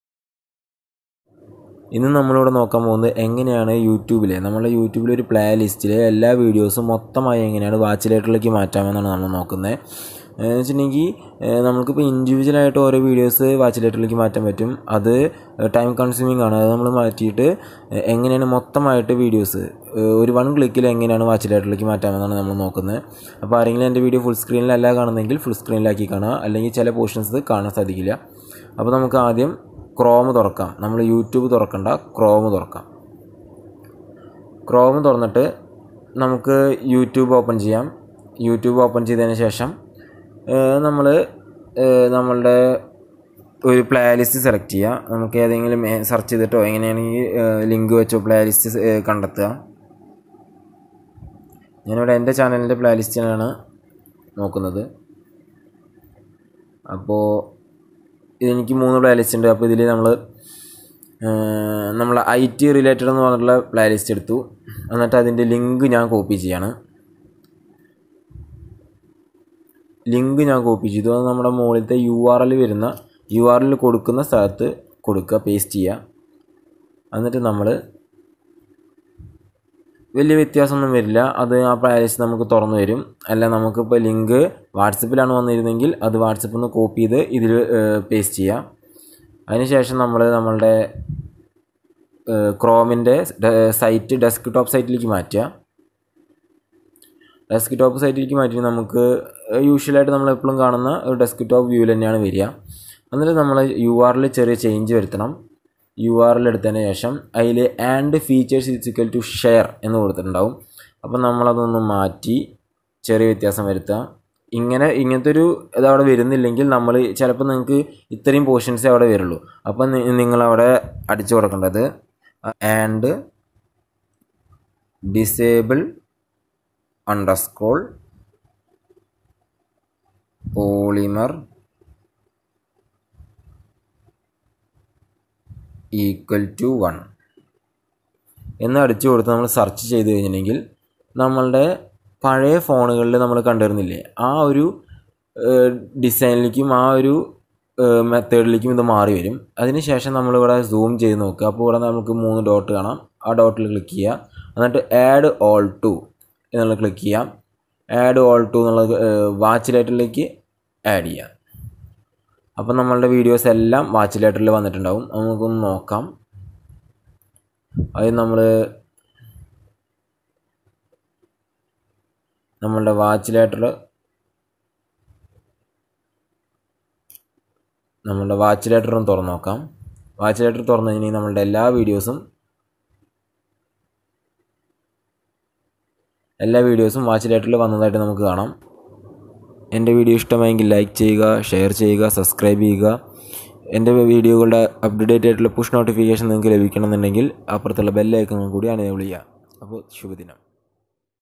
How to add all videos to Watch Later in one click from playlist | Malayalam
paste at end of your URL

&disable_polymer=1